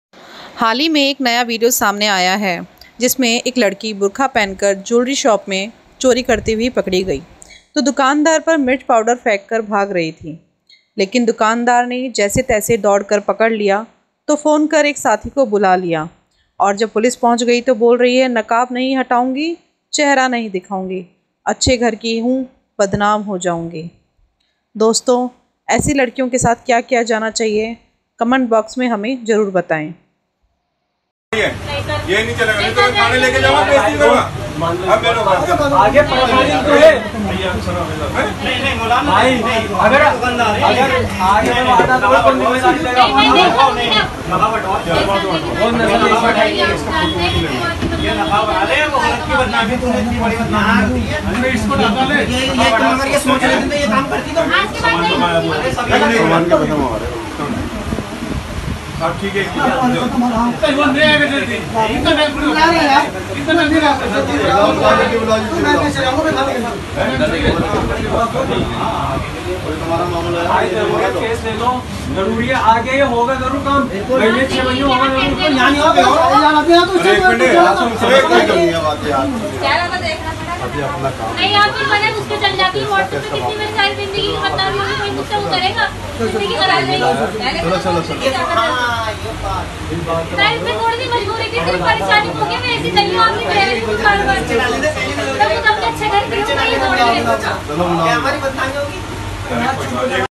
indian girl caught stealing in jewellery shop wearing burkha बुर्का पहनकर लड़की ने की चोरीburkha
indian girl caught stealing in jewellery shop wearing burkha बुर्का पहनकर लड़की ने की चोरी

बुरका पहन कर जेवलरी दुकान में चोरी करती पकड़ी गई तो दुकानदार पर मिर्च पाउडर फेक कर भाग रही थी, लेकिन दुकानदार ने जैसे तैसे दौड़ कर पकड़ लिया तो फोन कर एक साथी को बुला लिया,और जब पुलिस पहुंच गई तो बोल रही है नकाब नही हटाऊंगी चेहरा नही दिखाउंगी अच्छे घर की हूँ बदनाम हो जाऊंगी !